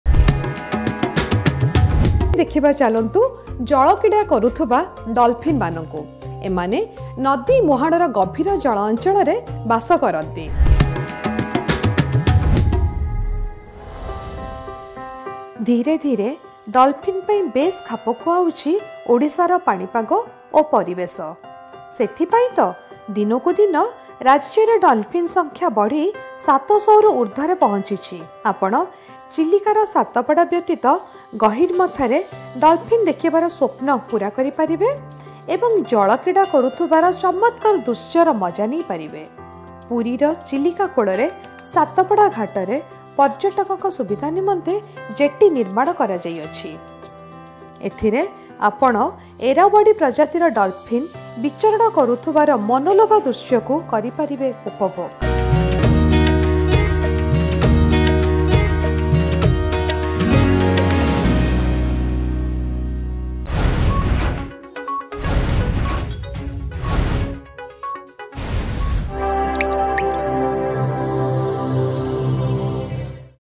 Ae Ama Sundara Odisha | ଓଡ଼ିଶାର ପାଣିପାଗ ପରିବେଶ ଡଲଫିନ୍‌ଙ୍କ ପାଇଁ ଖାପ ଖୁଆଉଛି | Dolphins
ଧିରେ ଧିରେ ଓଡ଼ିଶାର ପାଣିପାଗ ପରିବେଶ ଡଲଫିନ୍‌ଙ୍କ ପାଇଁ ଖାପ ଖୁଆଉଛି। ସେଥିପାଇଁ ରାଜ୍ଯରେ ଡଲଫିନ୍‌ ସଂଖ୍ୟା ବଢ଼ି ୭୦୦ରେ ପହଞ୍ଚିଛି...।